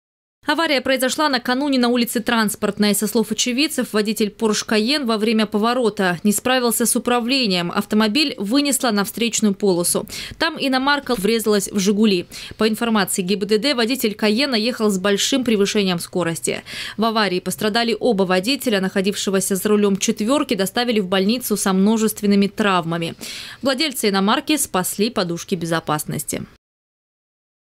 В Сочи водитель дорогой иномарки протаранил «Жигули». Новости Эфкате
«Porsche Cayenne» на скорости врезался в ВАЗовскую «четвёрку». Причём на встречной полосе.

Авария произошла накануне на улице Транспортной. Со слов очевидцев, водитель «Porsche Cayenne» во время поворота не справился с управлением — автомобиль вынесло на встречную полосу. Там иномарка врезалась в «Жигули». По информации ГИБДД, водитель Кайена ехал с большим превышением скорости. В аварии пострадали оба водителя. Находившегося за рулём «четверки» доставили в больницу со множественными травмами. Владельца иномарки спасли подушки безопасности.
Сочи. Новости.
Телеканал Эфкате.